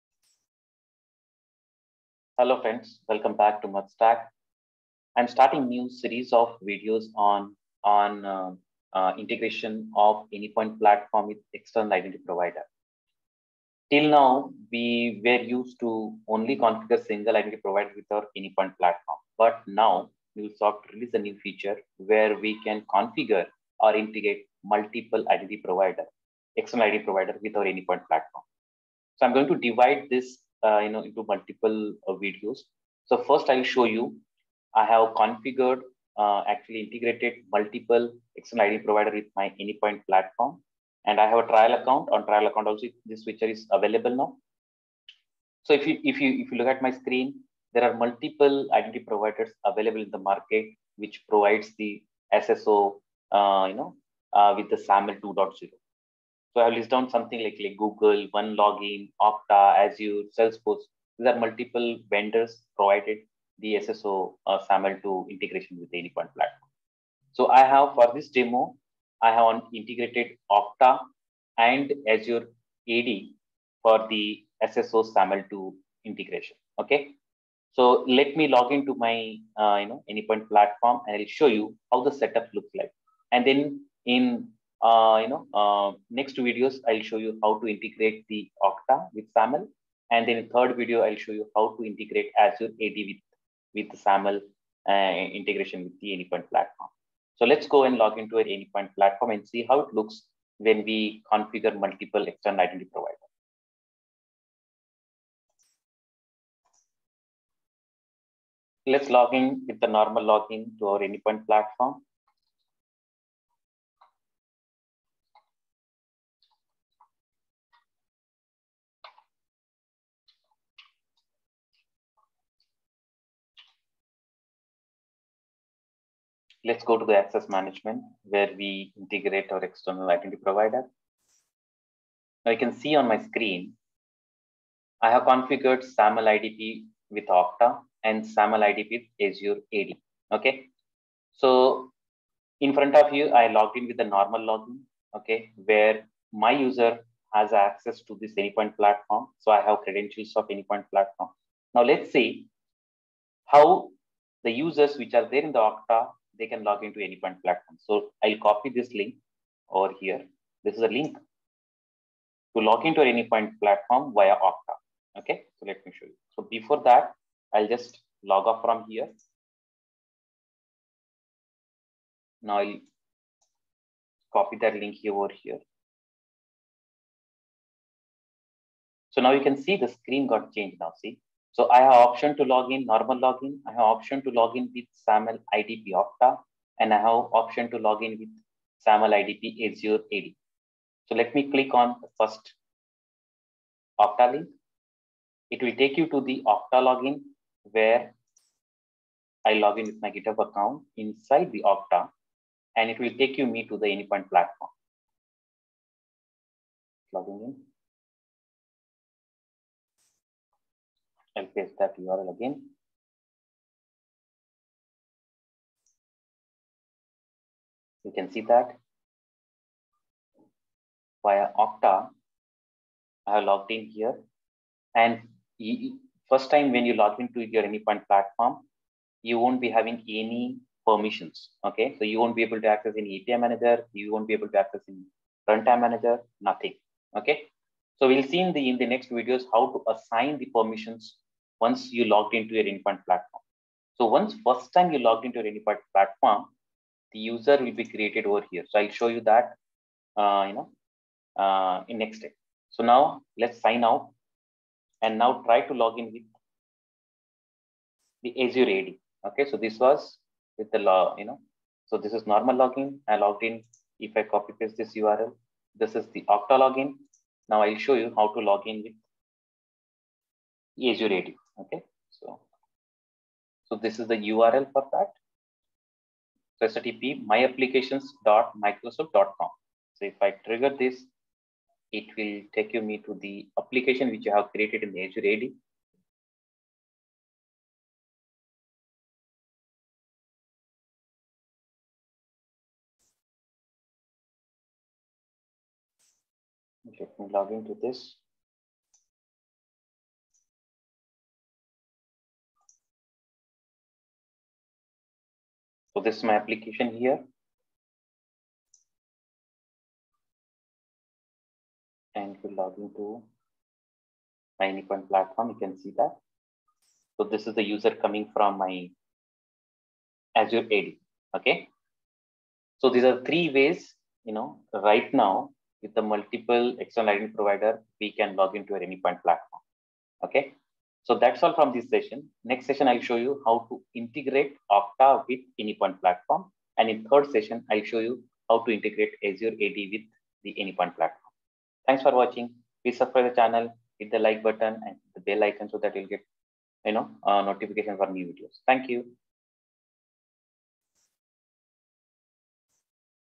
#1: Multiple External Identity Provider Integrations with Anypoint Platform | SAML 2.0 Overview
#1: Multiple External Identity Provider Integrations with Anypoint Platform | SAML 2.0: Technical Overview | SAML authentication | What is single sign on?

What you'll learn from this Session:
- How to configure Multiple External Identity Provider Integrations with Anypoint Platform?
- How SAML authentication works?

SAML stands for Security Assertion Markup Language. It is an XML-based open-standard for transferring identity data between two parties: an identity provider (IdP) and a service provider (SP).

Identity Provider — Performs authentication and passes the user's identity and authorization level to the service provider.

Service Provider — Trusts the identity provider and authorizes the given user to access the requested resource.